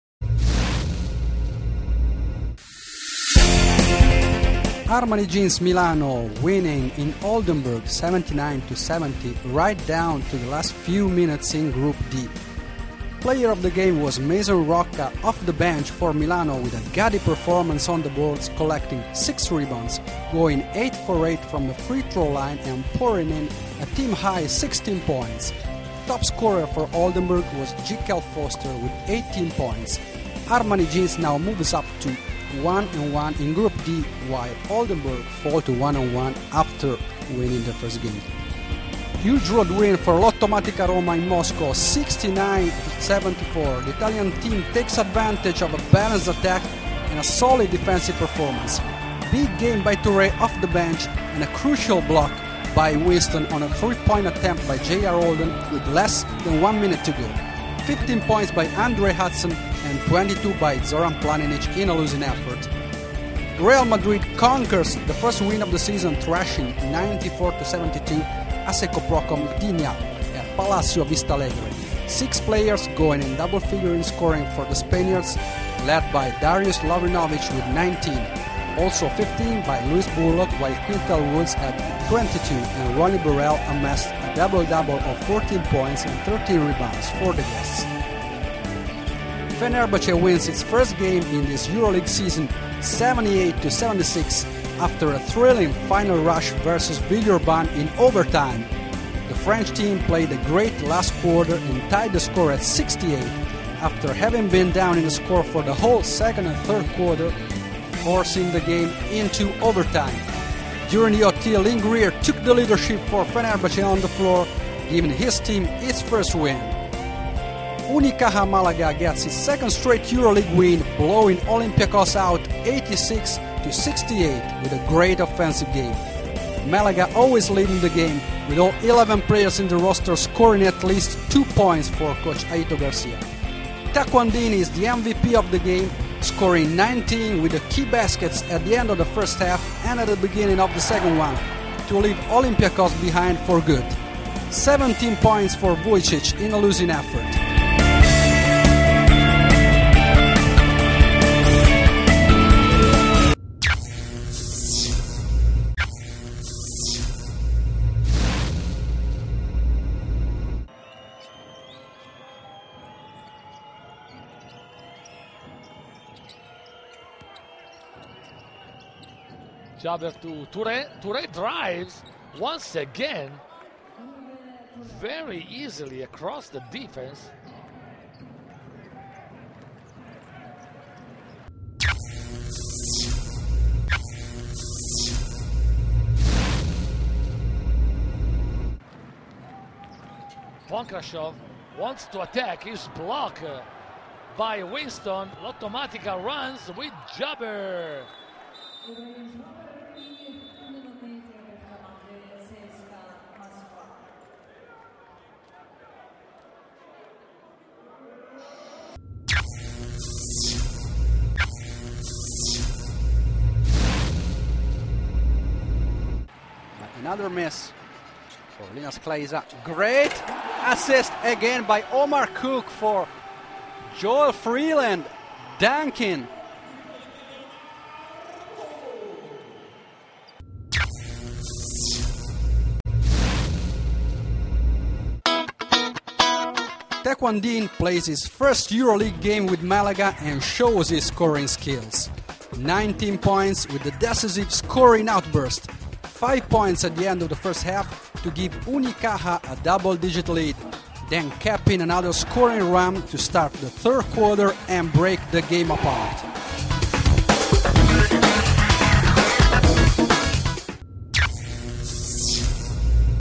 Euroleague 2010 - Week 2 - Gameday 1 Review (28-10-09) Dunk - Block - Assist of the Night
Euroleague 2010 - Week 2 - Regular Season - Gameday 1 Review (28-10-09) Dunk - Block - Assist of the Night